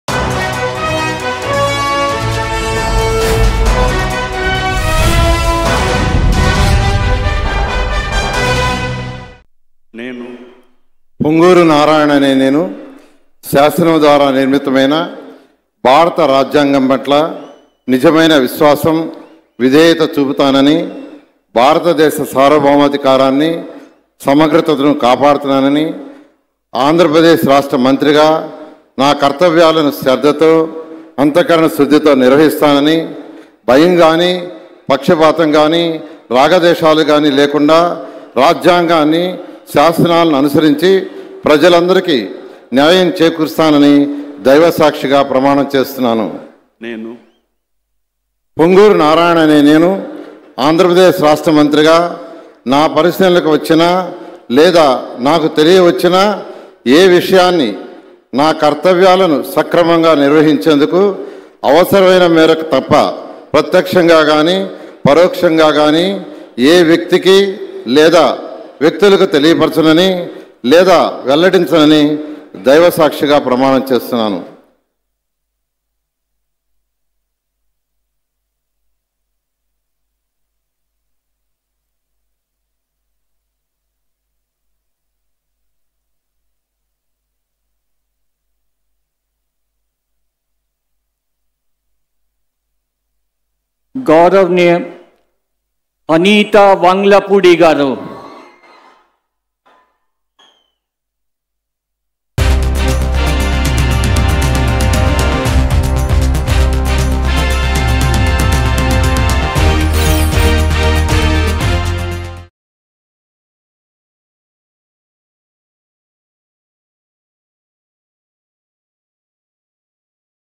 Ponguru Narayana Takes Oath As AP Cabinet Minister | AP Cabinet Ministers 2024 Swearing In Ceremony
Ponguru Narayana Takes Oath As AP Cabinet Minister. Nara Chandrababu Naidu took oath as the Chief Minister of Andhra Pradesh at Kesarapalli IT Park, Gannavaram, Krishna District. The ceremony was administered by AP Governor Syed Abdul Nazeer. Chandrababu, along with other members of the AP Cabinet Ministers, also took the oath.